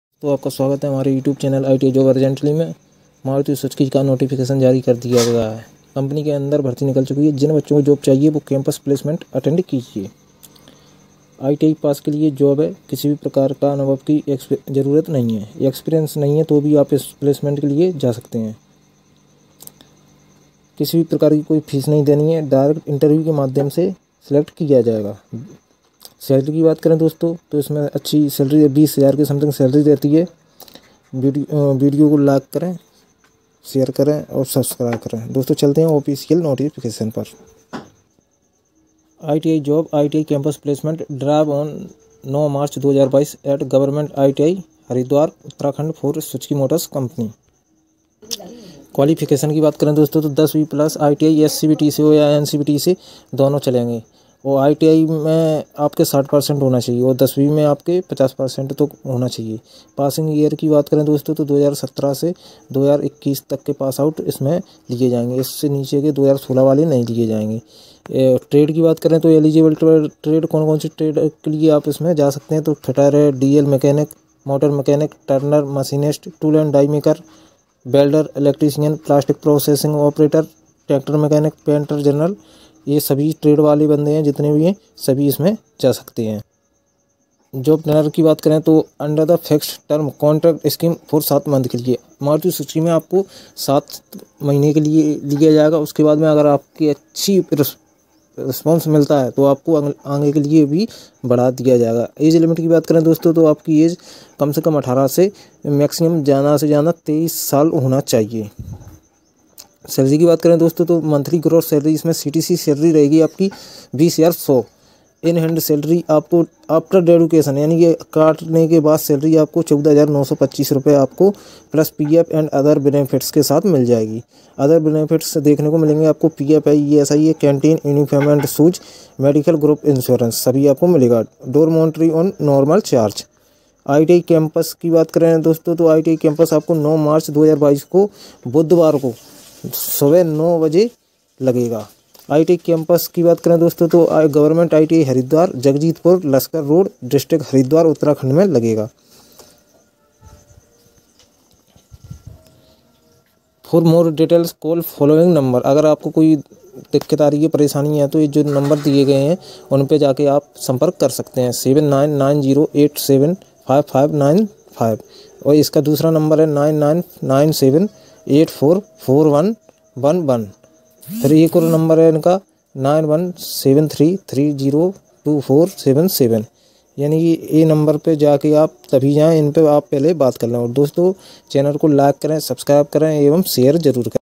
Maruti Suzuki India limited campus placement at haridwar
maruti Suzuki campus placement urgent job requirements at haridwar uttarakhand
iti passout all trade apply